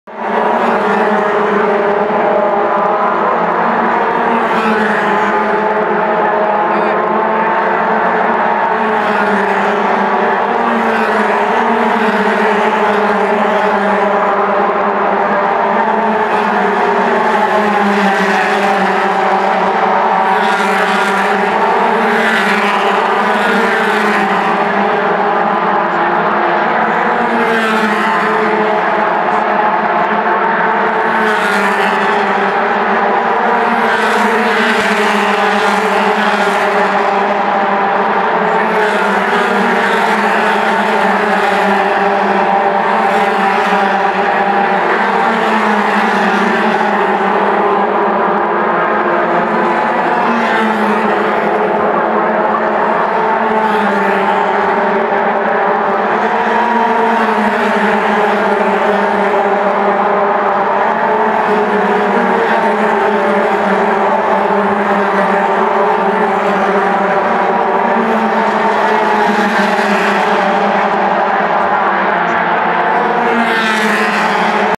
Allied Building Products US F2000 Oval Challenge - View From Turn 4
Another video of a similar position, but I had physically moved closer to turn 4 at this point.